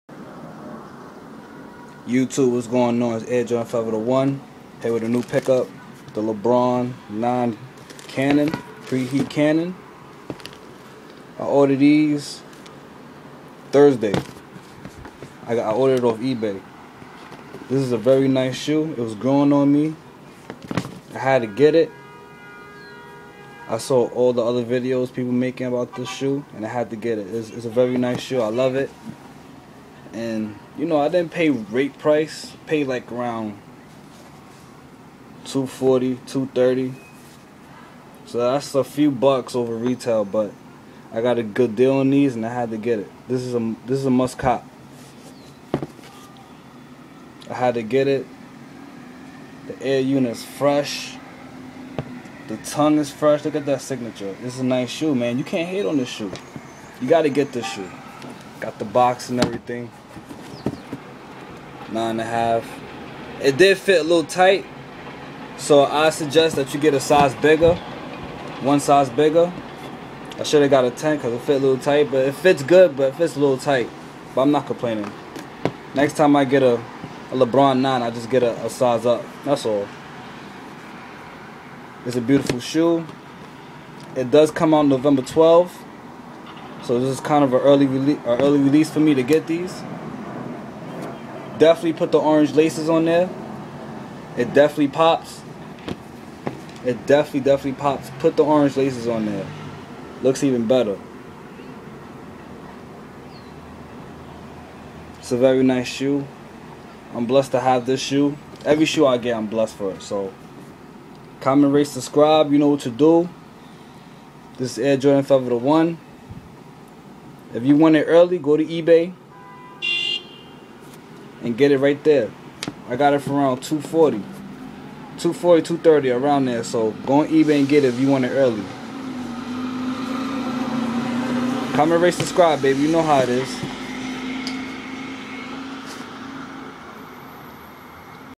Early release Nike lebron 9 pre-heat cannon
Cop this shoe off eBay very nice shoe this is a must cop for any sneaker head old or young don't miss out on these release date November 12 show your love and stay safe